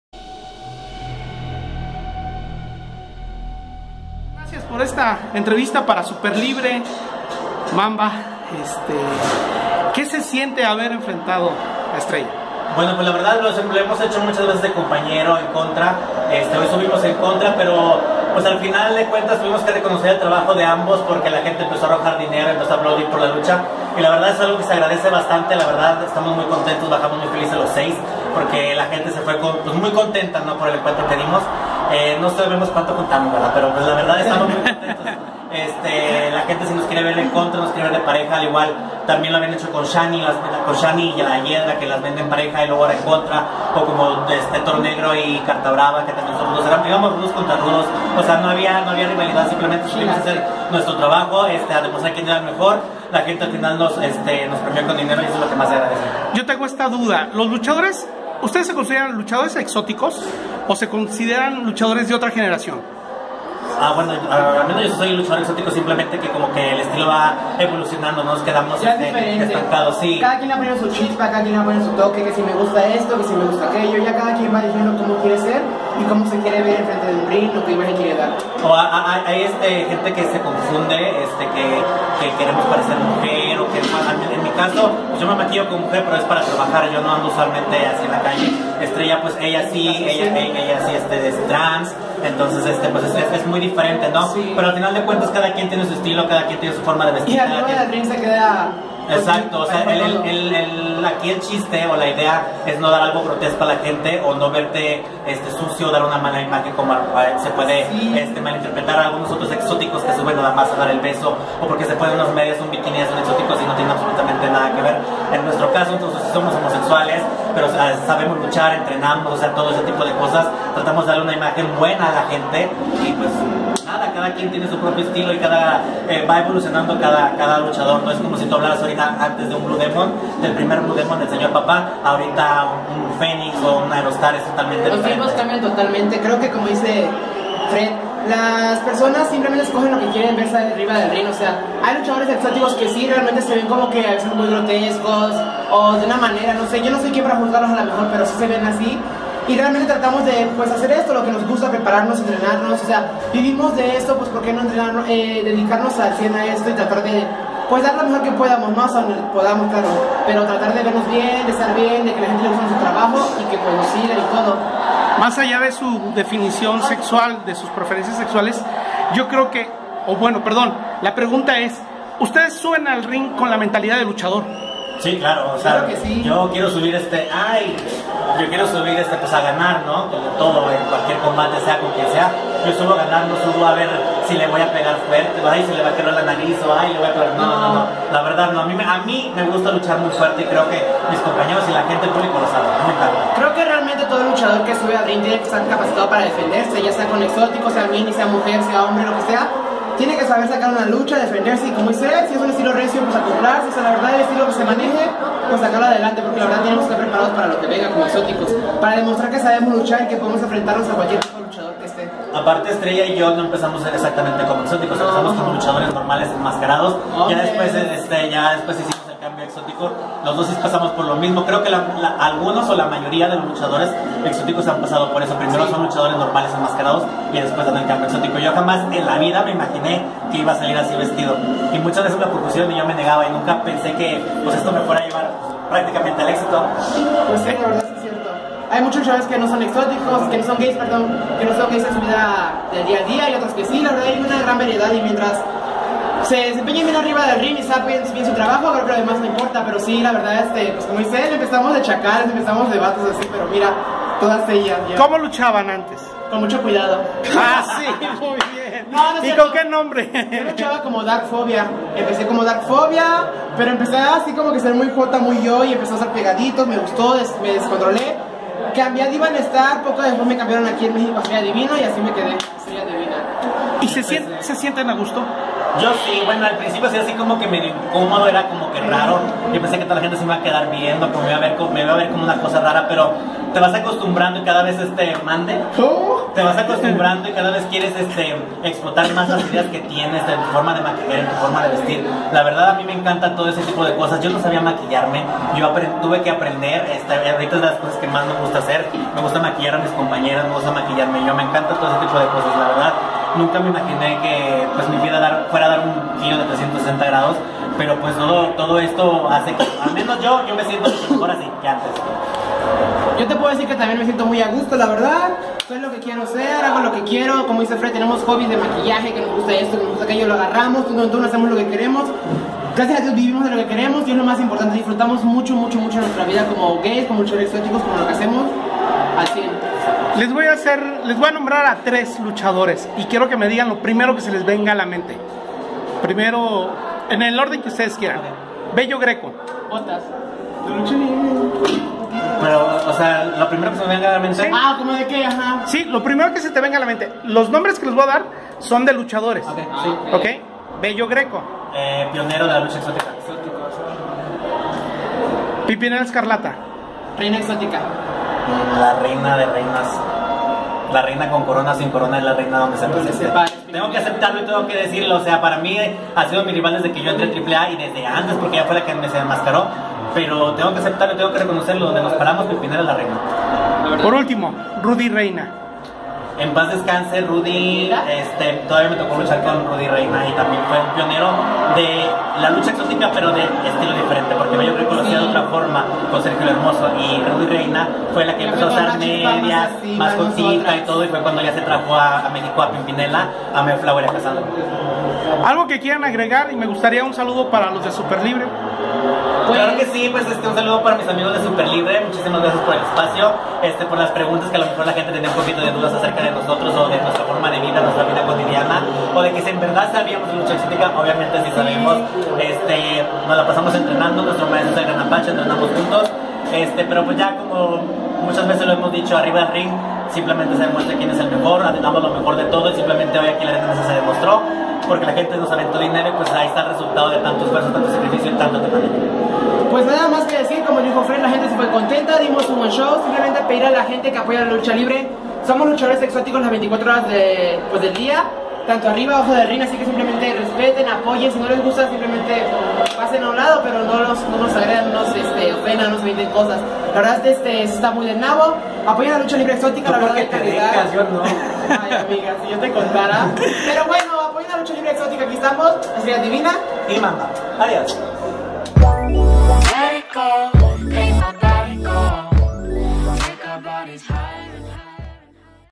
MAMBA y ESTRELLA DIVINA más que una entrevista !
Una entrevista que se convirtió en una platica llena de anécdotas, confesiones y divertidos comentarios... tienen que verla completa !